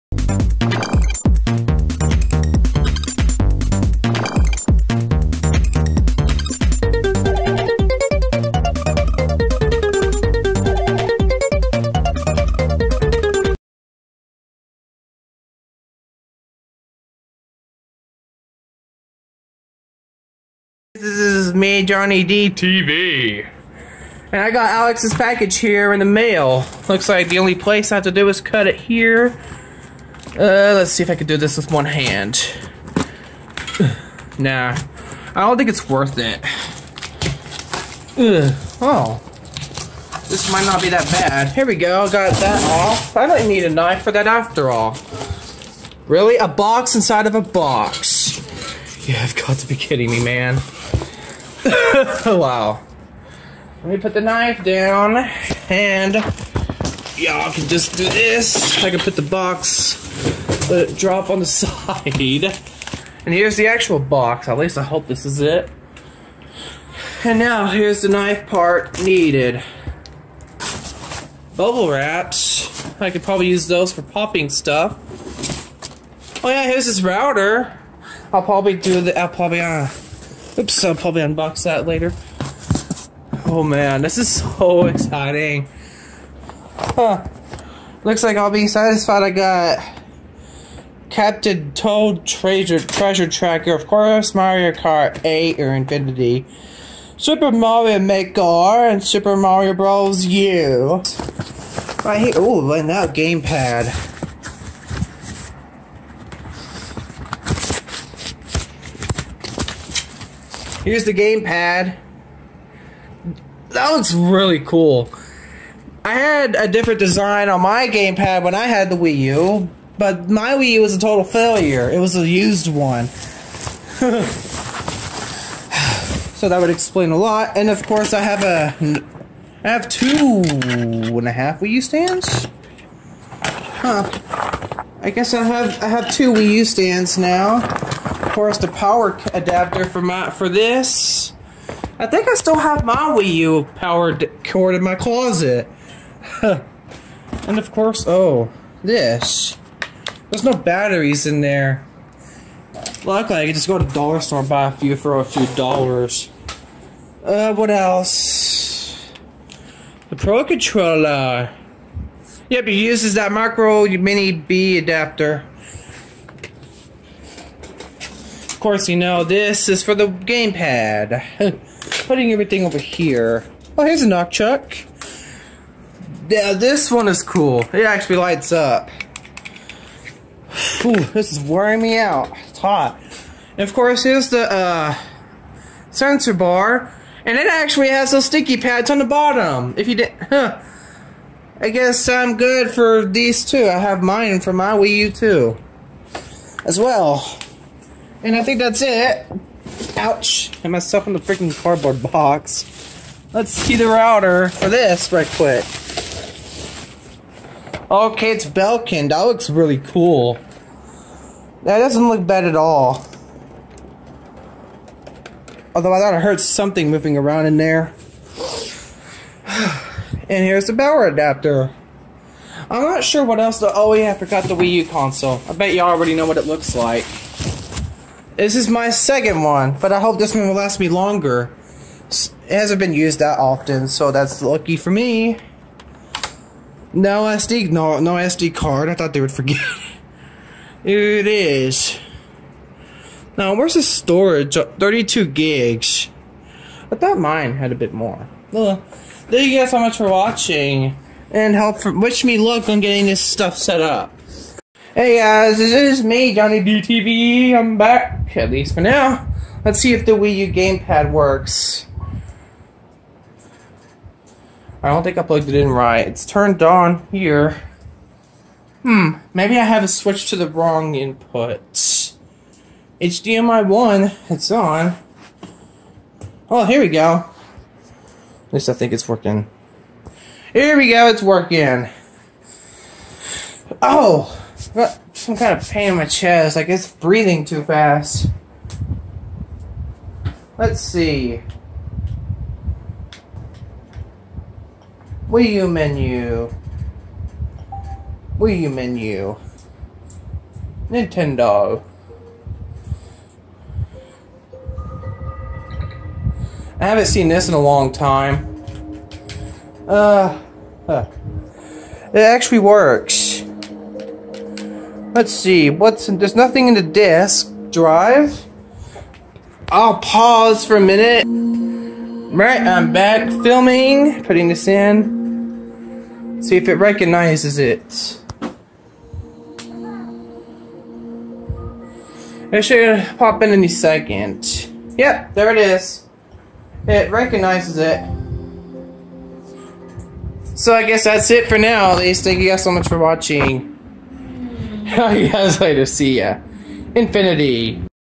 Unboxing a Wii U.
For my birthday my friend sent me a free console and something else watch the video to find out.